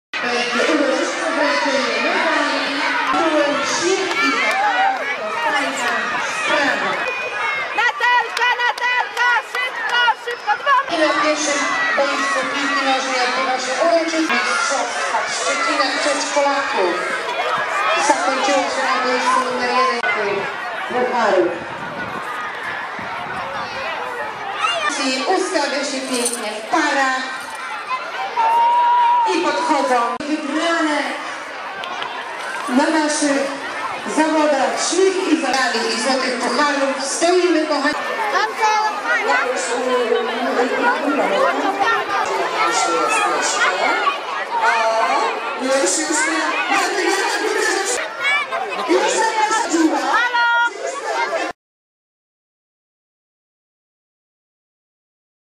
Szczecin: "Śmiech i zabawa to fajna sprawa"
Przedszkolaki rywalizowały na stadionie lekkoatletycznym w dziesięcioboju sportowym. Były medale, puchary i uśmiechy dzieci.